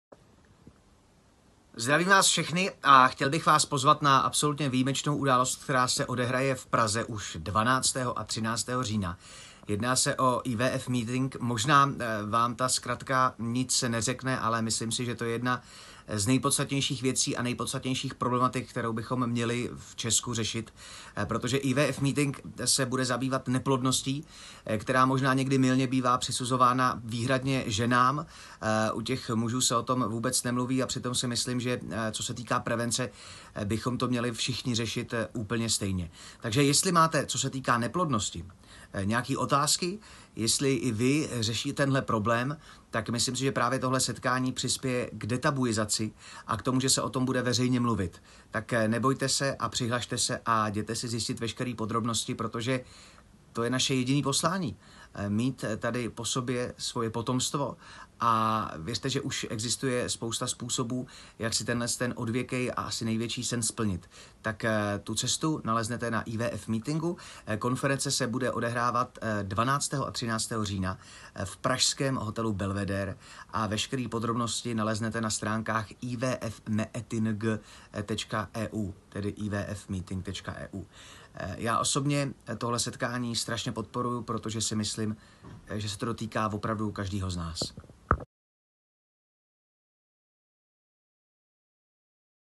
Libor Bouček Vás zve na IVF meeting Prague 2019 - konferenci o (ne)PLODNOSTI
Libor Bouček je nejen úžasný moderátor, ale hlavně člověk se velkým srdcem a my si velice vážíme i jeho podpory našeho projektu IVF meeting Prague 2O19. Neplodnost je stále velké tabu. To chceme změnit! Ptejte se přímo odborníků na vše, na co jste se dosud báli zeptat. Možná Vám to přinese odpovědi, na které dlouho čekáte. Potkejte specialisty na neplodnost i podporu plodnosti a zkraťte a zpříjemněte si cestu k miminku.  Jsme tu pro Vás! Počet míst na konferenci je bohužel omezený, neváhejte a zarezervujte si své místo na